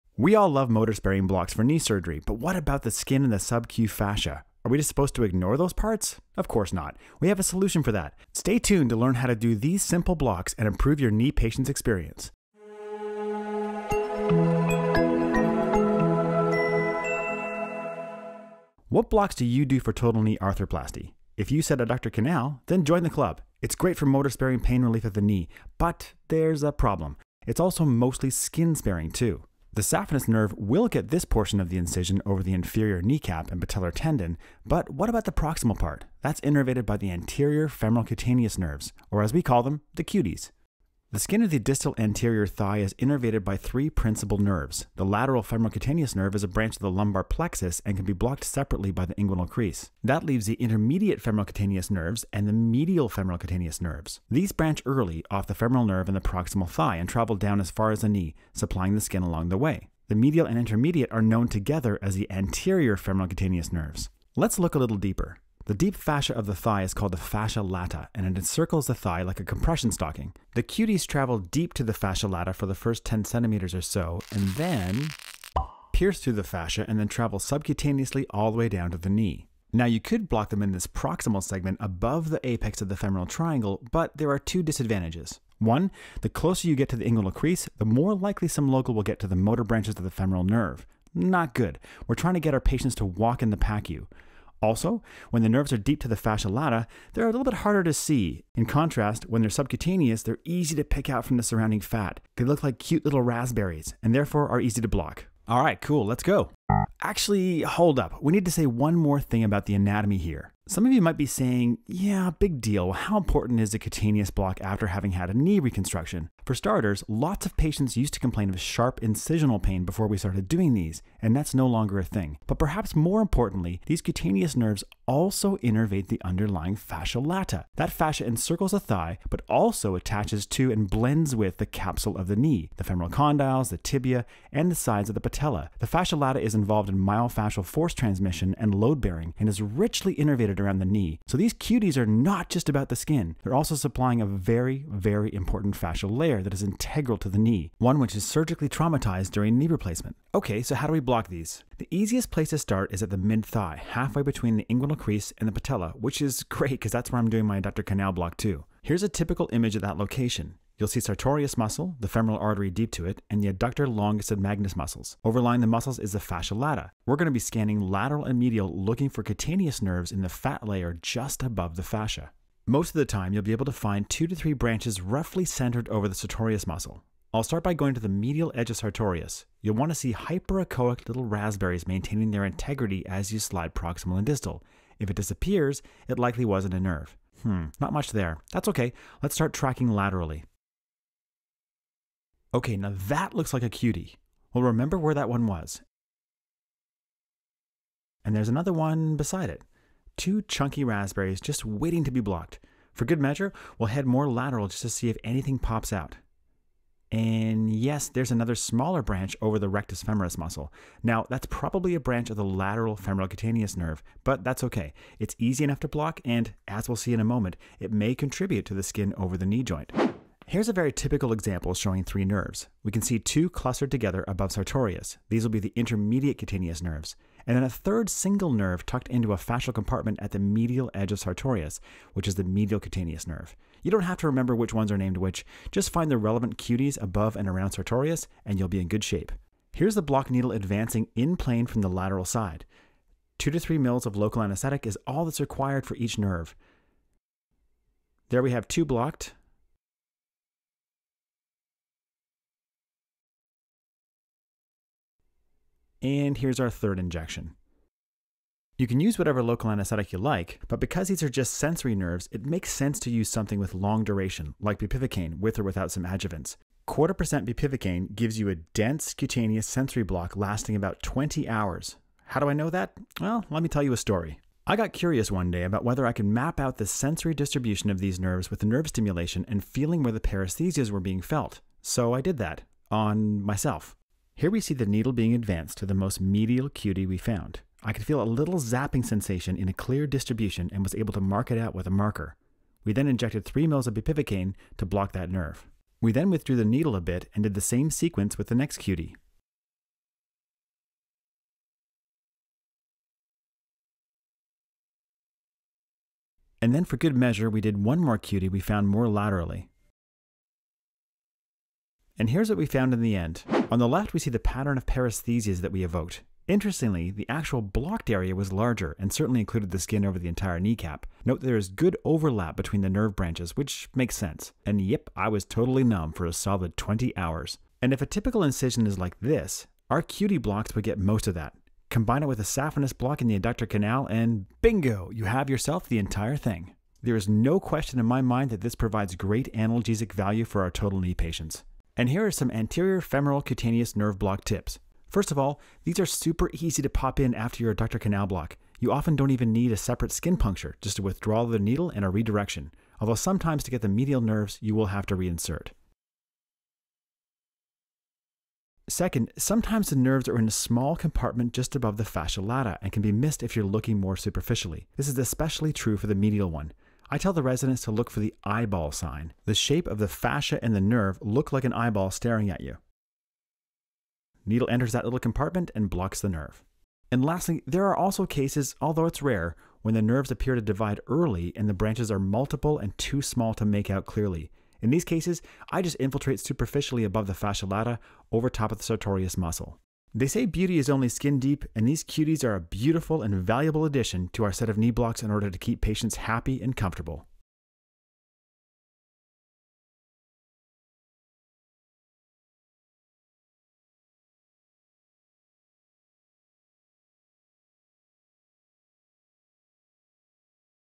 Anterior Femoral Cutaneous Nerve Block (The Cuties!)
In this video, we describe the anatomy, sonoanatomy and technique for blocking the anterior femoral cutaneous nerves, which is extremely useful for pain control after knee replacement and other knee procedures.